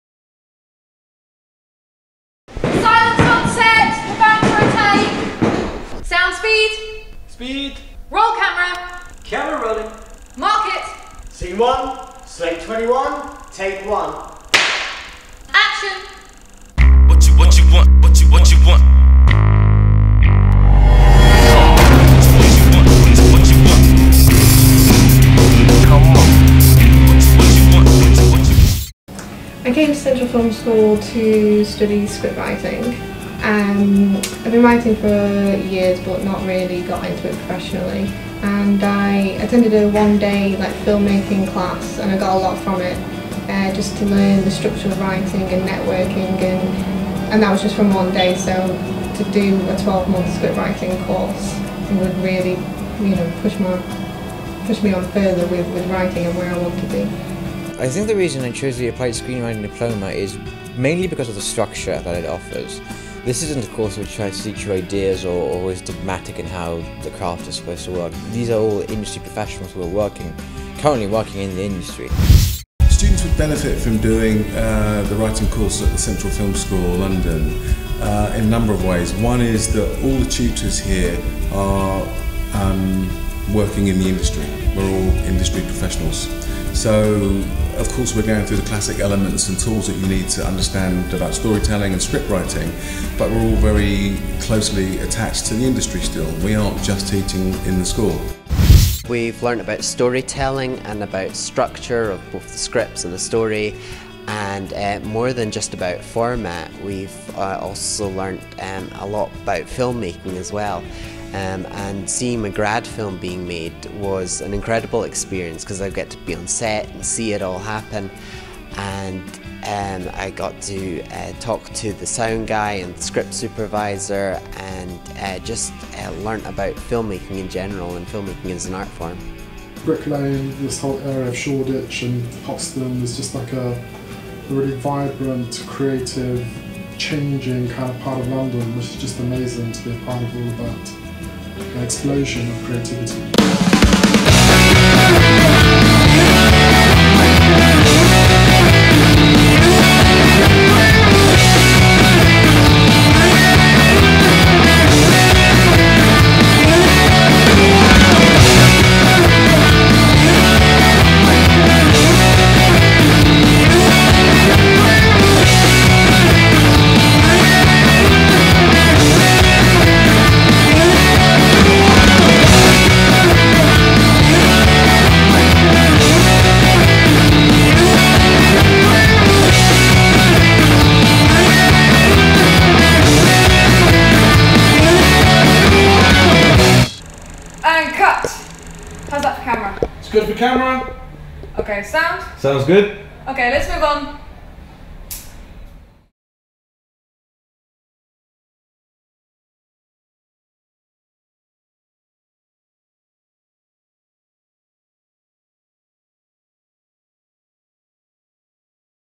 Full-Time Screenwriting Course Video - Central Film School London 2012
A snapshot of life at Central Film School London studying filmmaking on the Full Time One Year Applied Screenwriting Diploma course.